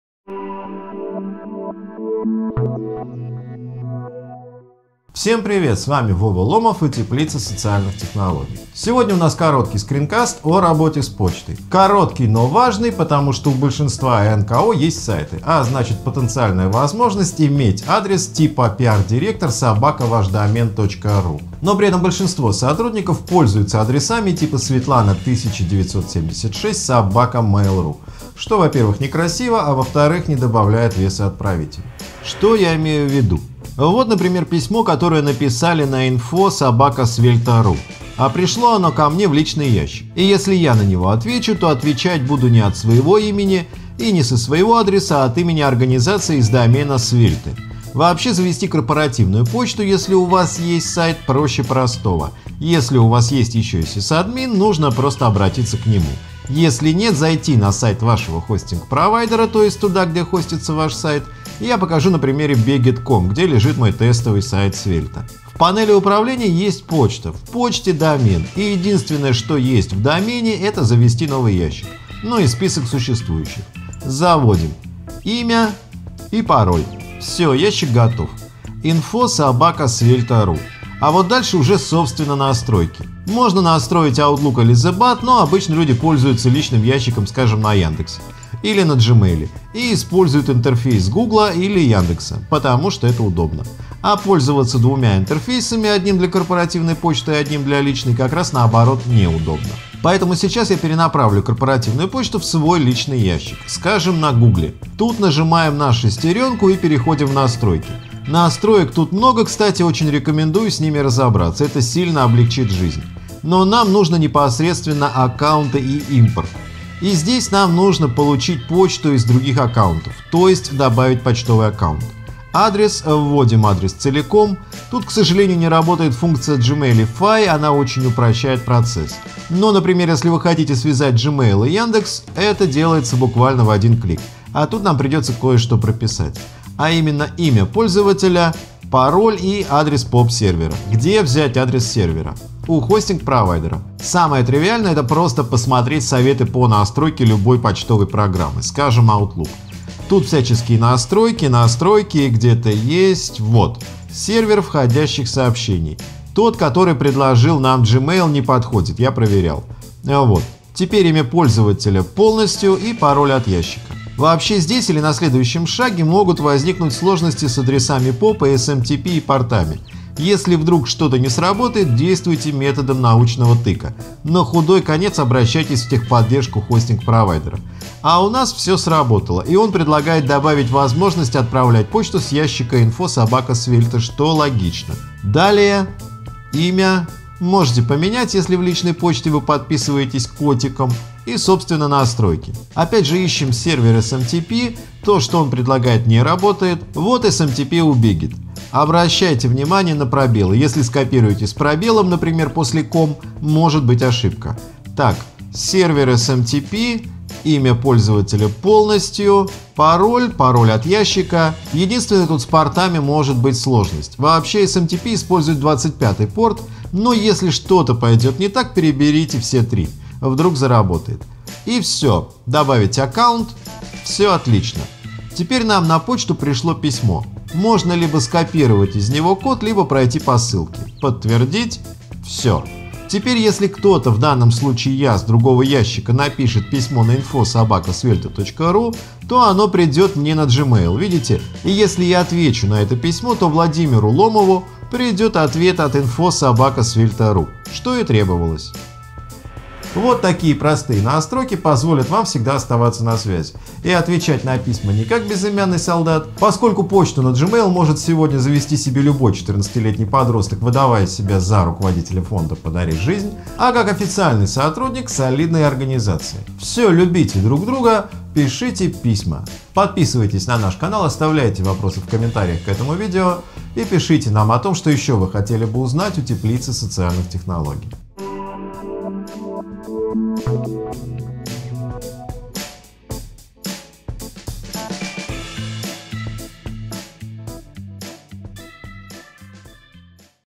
Как настроить пересылку корпоративной почты на личный адрес
Практически у любой НКО есть сайт, а значит у сотрудников есть потенциальная возможность иметь корпоративную почту типа . Но при этом большинство сотрудников НКО используют бесплатные почтовые сервисы и адреса типа , что, конечно, не добавляет веса отправителю и негативно сказывается на имидже организации.

Понятно, что происходит это потому что у каждого человека есть своя личная почта и работать с разными адресами в разных сервисах просто неудобно, но эту проблему можно решить достаточно просто — перенаправить почту с корпоративного ящика на свой личный. Таким образом в одном интерфейсе можно получать письма, пришедшие на разные адреса и  и отвечать на них с того адреса, на который вам писали.

В нашем видео мы рассказываем как это сделать на примере почтового сервиса от Google, но в принципе настройки Яндекс почты не многим отличаются от Gmail. А заодно из нашего видео вы узнаете, как завести себе корпоративной  адрес.

Видео на Теплице - это уроки на русском по всем программам, которые могут вам пригодиться.